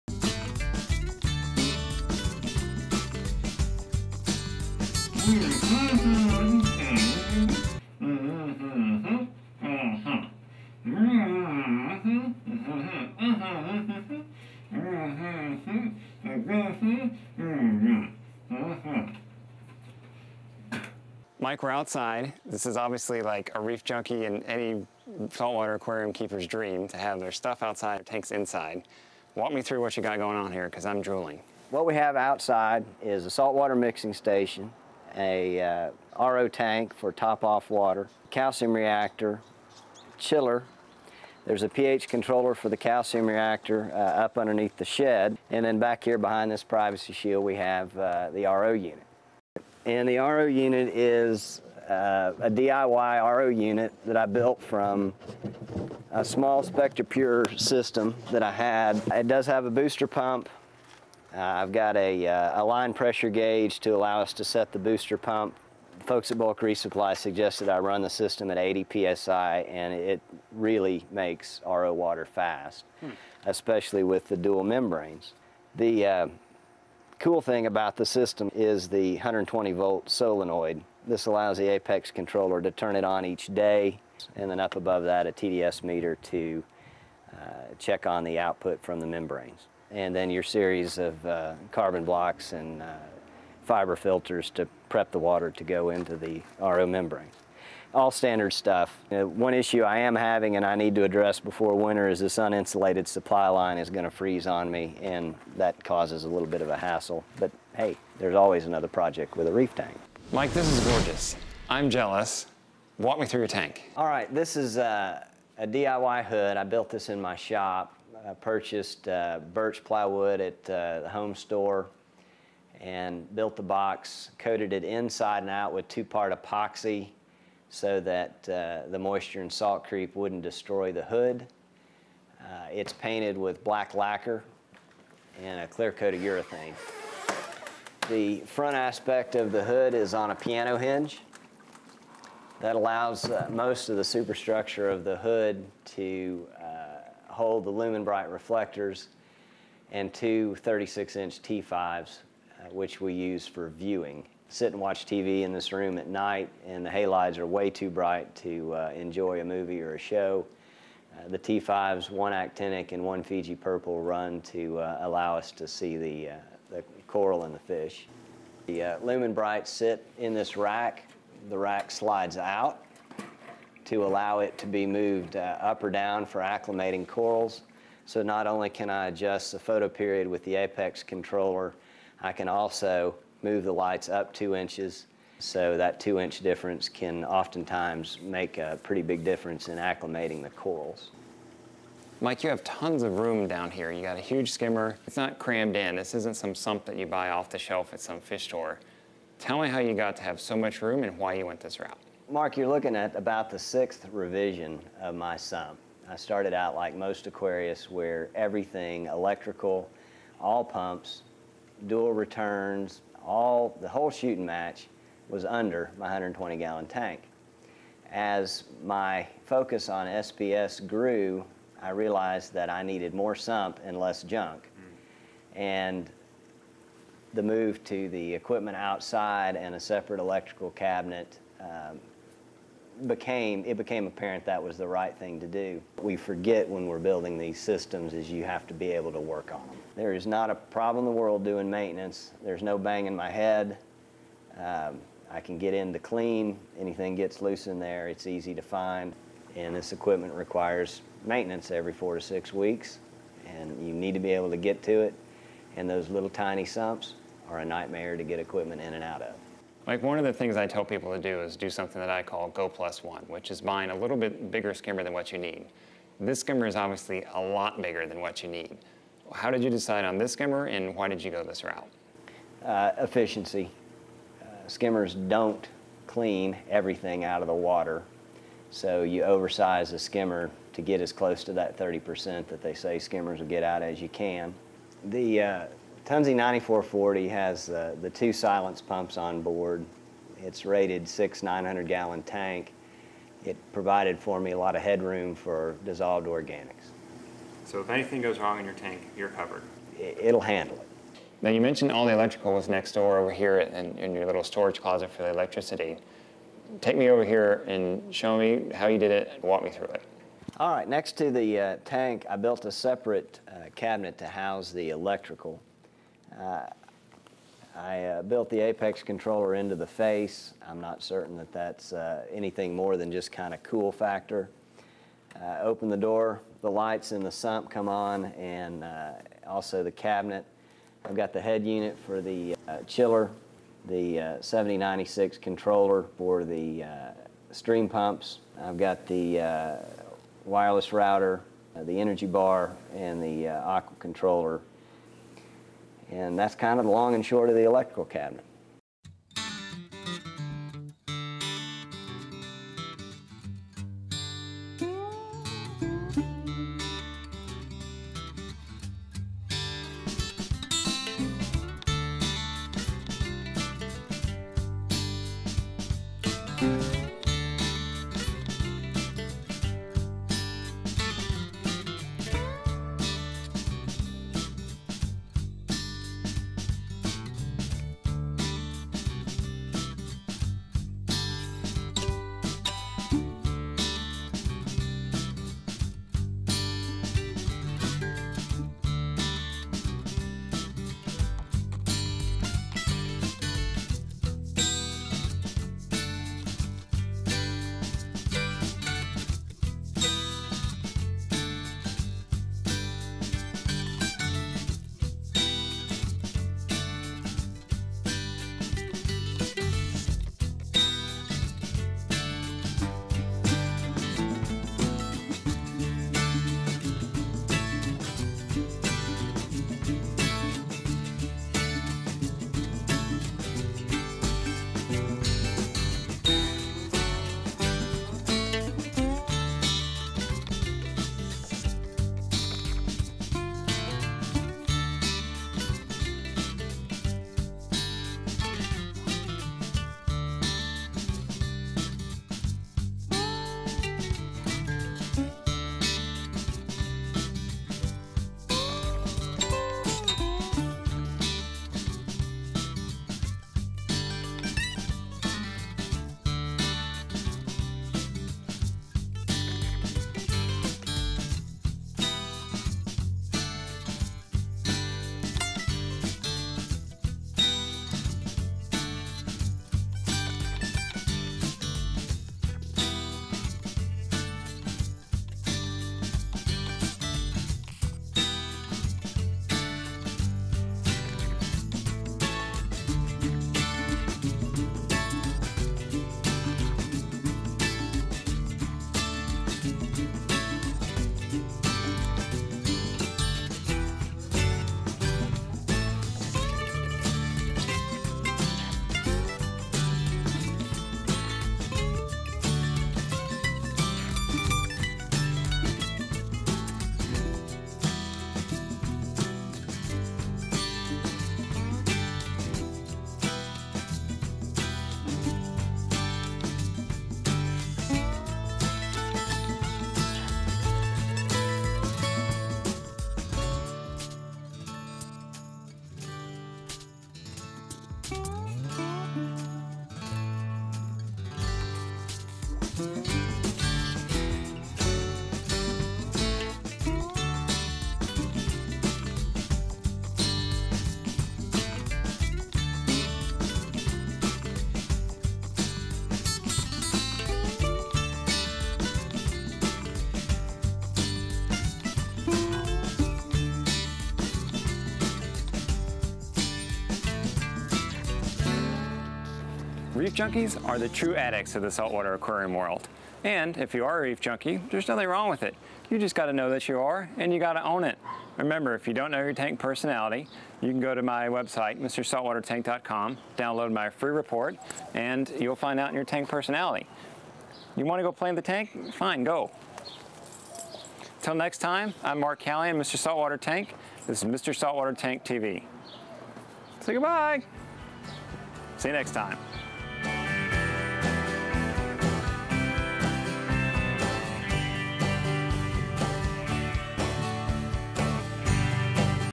A Reef Junkie Profile
Reef Junkie's life revolves around their tank. The tank is part of the family and they are proud of it.

In this episode of Mr. Saltwater Tank TV, I go to my friend Mike's house, a die-hard reef junkie, to check out his 120G tank and all the glory that goes with it